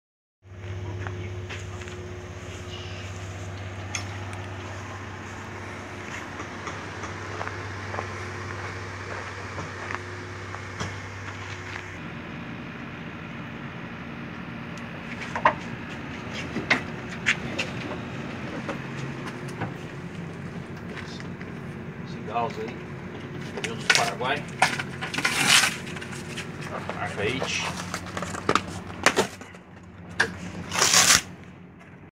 PRF apreende 400 mil maços de cigarros contrabandeados do Paraguai
Confira os nosso vídeos sobre Turismo, Política, Esportes, entretenimento, Saúde, Reportagens e Matérias especiais no Mato Grosso do Sul e fora do Estado. - Fonte do Vídeo: PRF/MS